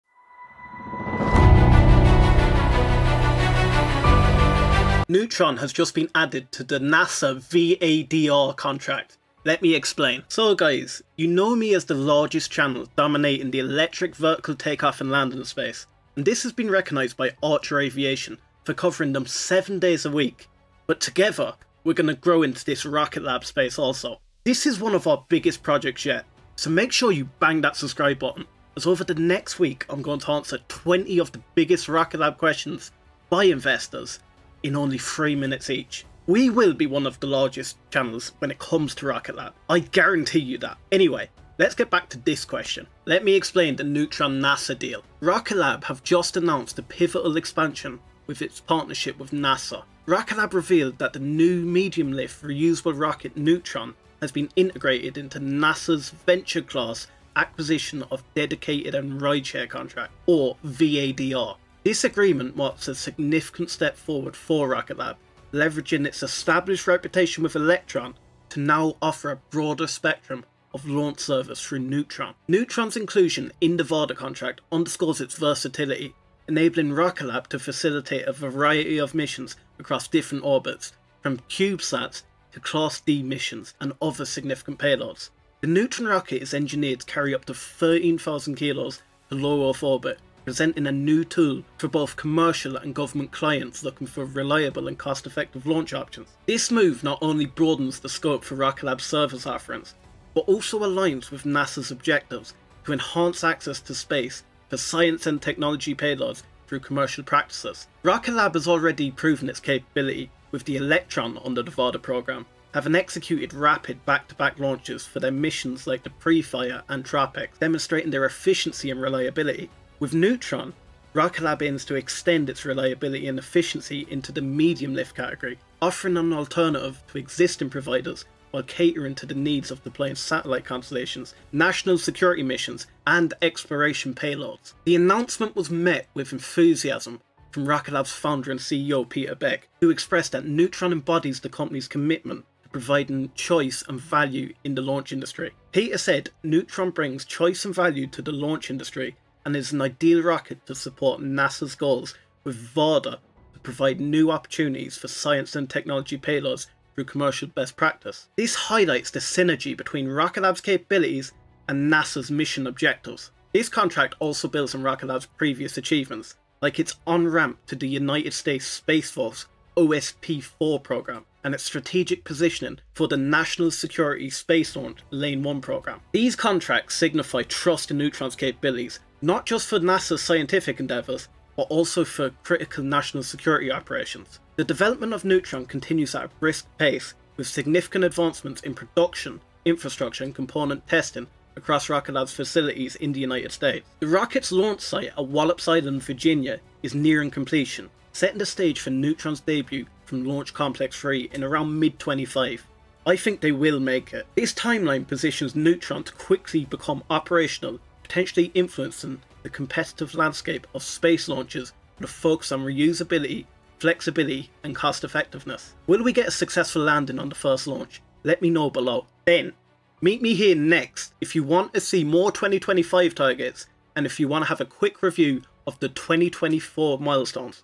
Rocket Lab Neutron SELECTED for NASA VADR Contract!
Witness a significant milestone in the space industry as Rocket Lab announces the inclusion of its Neutron rocket into NASA's Venture-Class Acquisition of Dedicated and Rideshare (VADR) contract. From its headquarters in Long Beach, California, Rocket Lab sets a new course for its future, extending its expertise beyond the Electron rocket to introduce a versatile, medium-lift option for various space missions. Join us as we delve into what this means for Rocket Lab, NASA, and the broader space community.

Rocket Lab's Strategic Expansion with Neutron Rocket Lab, a name synonymous with innovation in rocket technology, has once again proven its capability to evolve and meet growing market demands. The integration of the Neutron rocket into the VADR contract allows Rocket Lab to offer expanded launch services, facilitating a wide range of missions from deploying CubeSats to executing Class D missions and handling significant payloads. Neutron is designed to transport up to 13,000 kg to low Earth orbit, demonstrating Rocket Lab's commitment to providing cost-effective and reliable launch options for both commercial and government entities.

Neutron Rocket: A Game Changer for Medium-Lift Missions Neutron's role in NASA's VADR contract underlines its potential to change the dynamics of medium-lift space missions. Rocket Lab aims to build on the proven track record of the Electron rocket, which has already demonstrated swift and reliable launch capabilities under the VADR program through missions like PREFIRE and TROPICS. With Neutron, Rocket Lab is set to enhance its offerings, providing an efficient alternative to existing launch providers, ideal for satellite constellations, national security missions, and deep-space explorations.

Implications for NASA and Commercial Space Ventures The partnership with NASA through the VADR contract aligns with both organizations' goals to improve access to space with innovative and commercial practices. Neutron's versatility supports NASA's objectives to foster scientific and technological research, offering a reliable platform for future payloads. Rocket Lab's founder and CEO, Sir Peter Beck, emphasized Neutron's role in delivering choice and value to the launch industry, mirroring NASA’s ambitions to expand opportunities for science and technology initiatives.

Future Prospects and Industry Impact This development builds on Rocket Lab's previous successes, including its integration into the United States Space Force’s OSP-4 program and its positioning for the National Security Space Launch (NSSL) Lane 1 program. These achievements underline the industry's confidence in Neutron's capabilities, not just for NASA's scientific missions but also for crucial national security payloads. With significant advancements in its production and the nearing completion of its launch site at Wallops Island, Virginia, Neutron is on track for its debut launch in mid-2025, poised to reshape the competitive landscape of space launches.

Why This Video Is a Must-Watch If you're fascinated by the rapid advancements in space technology, or if you're closely following Rocket Lab's progress and its impact on space access, this video is for you. We break down the technical capabilities of the Neutron rocket, its strategic importance to NASA, and its potential to influence future space missions.